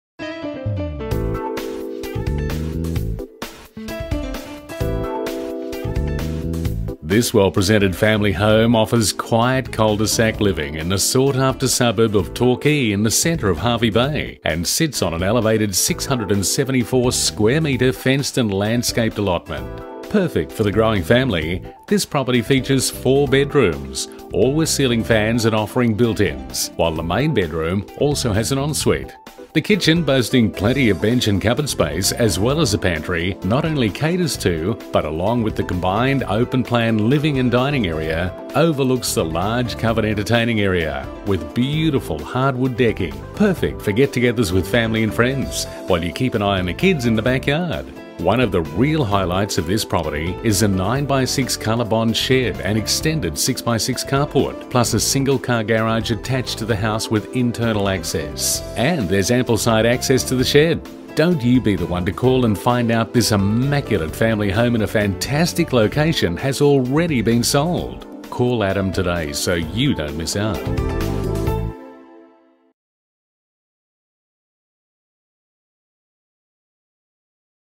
SOLD by ADAM KRATZMAN RE/MAX Partners Hervey Bay REAL ESTATE and Property Management Free Valuation
SOLD SOLD SOLD SOLD SOLD by Adam Kratzmann

11 Cleo Court, TORQUAY QLD 4655: Marketed by Adam Kratzmann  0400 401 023

Immaculate Family Home In A Fantastic Location

Located in the centre of Hervey Bay, in the sought after suburb of Torquay, is this well presented family home.  The property features four bedrooms all with built-ins and ensuite to main. The open plan living, dining and kitchen overlooks the large covered entertaining area with beautiful hardwood decking. One of the real highlights of this property is the 9x6 colorbond shed and extended 6x6 carport, plus a single car garage attached to the house with internal access.  Ample side access to the shed, and quiet cul de sac living. Situated on an elevated 674m2 fenced and landscaped allotment.
Price: $359,000
Agent: Adam Kratzmann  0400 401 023
Real Estate: Remax Partners Hervey Bay